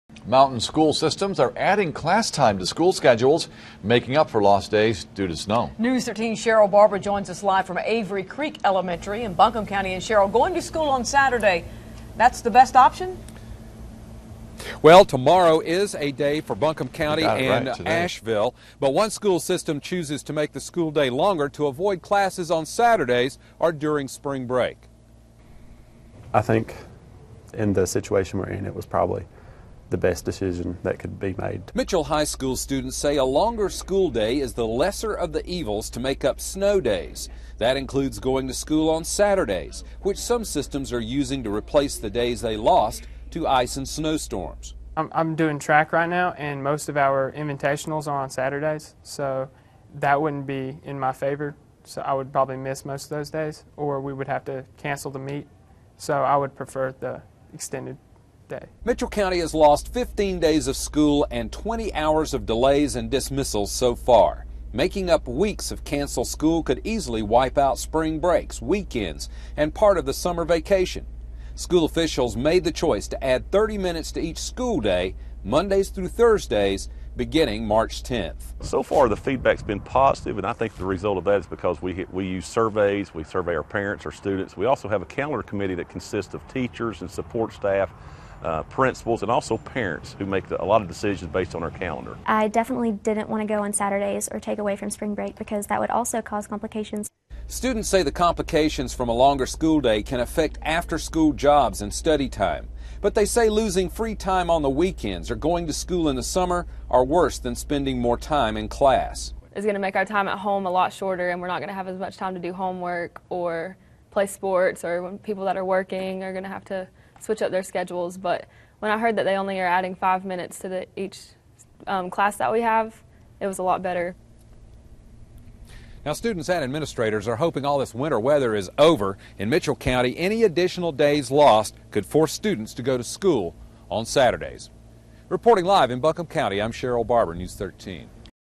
Making Up Snow Days
Last week's winter weather has forced Saturday classes and longer school days.

Mitchell County surveyed parents and students and found out they preferred to lengthen the school day to make up all the snow days. Mitchell County lost 15 days to bitter cold temperatures and days of snowfall. Parents and students rejected Saturday classes and using up days in Spring Break to replace the lost days.

Beginning March 10th, school officials will add 30 minutes to each school day Mondays through Thursdays. That adds about five minutes to each class here at mitchell high school.

"We're hoping for warmer and dryer weather, that's what we're all crossing our fingers about, but I think with the plan we have in place, is the best we can do for our students and providing the most instructional time in our classrooms as well," said superintendent Dr. Rick Spurling.

School officials say it's much more difficult to add school days to the end of the year. That would force school leaders to re-schedule testing and graduation. There's one hitch in the longer school day plan. If Mitchell County is hit with another snowstorm, students could have to make up those days in Saturday sessions.